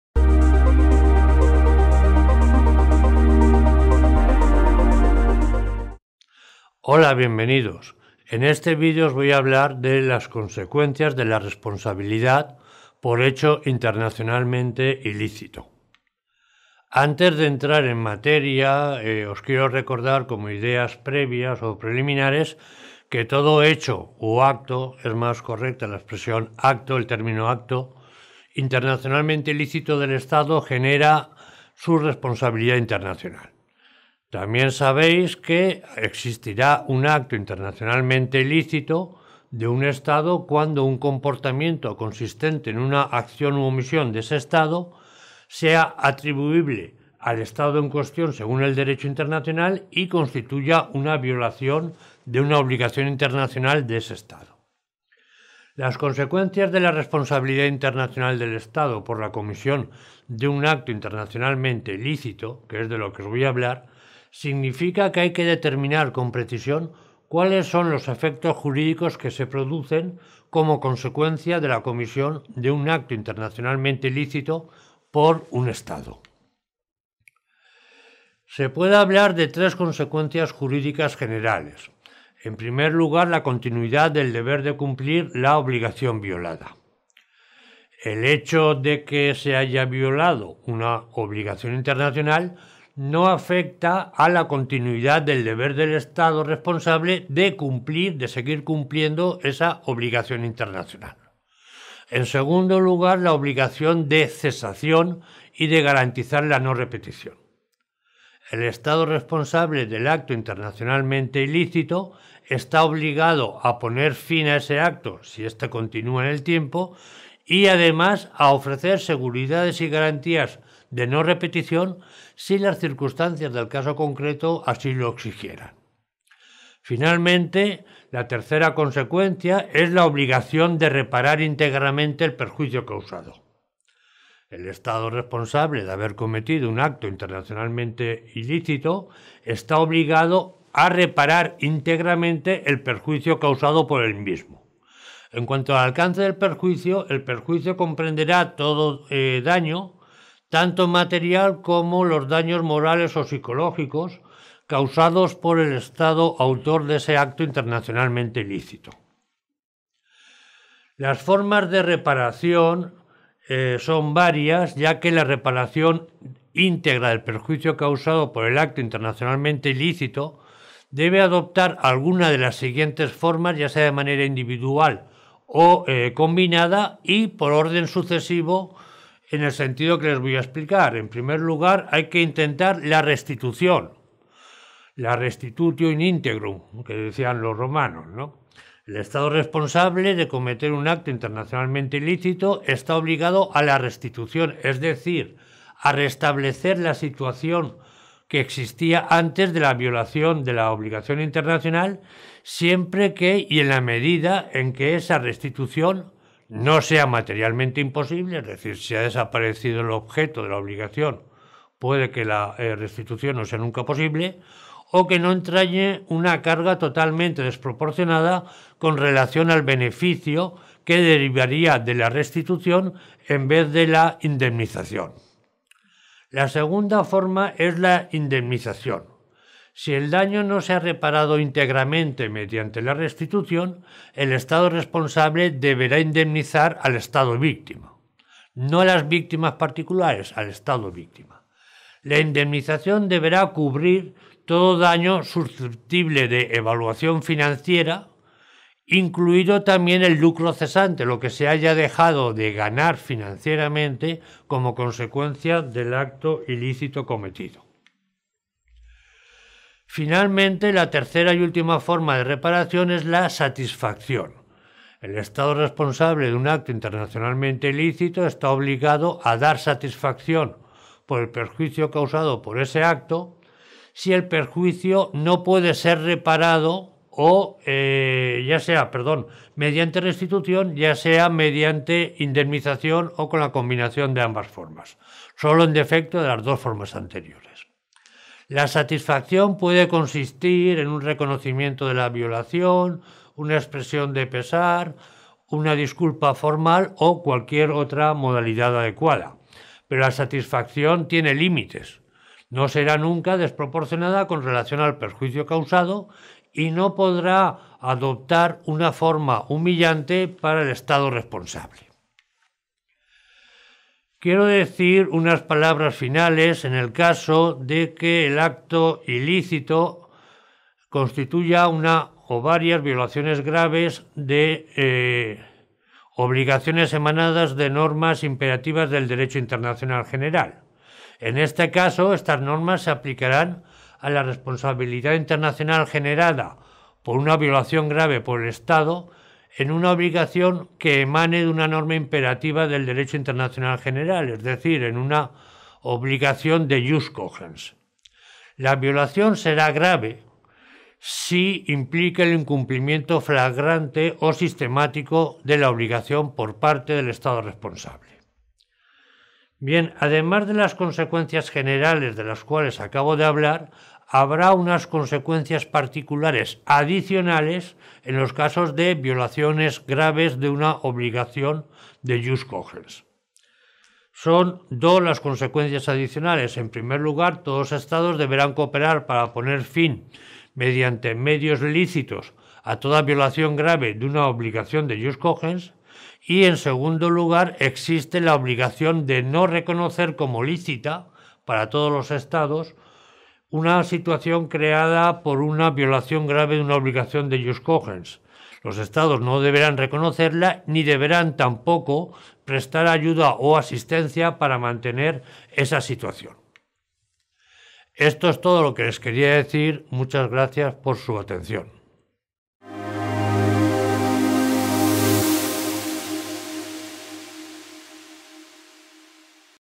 Consecuencias de la responsabilidad por hecho internacionalmente ilícito
Autor: Valentín Bou Franch
Serie: Derecho Internacional Público
Año: 2021
Resumen: En esta píldora educativa se explica la institución de la responsabilidad internacional en los casos de hechos internacionalmente ilícitos, sus requisitos, consecuencias y modos de resolución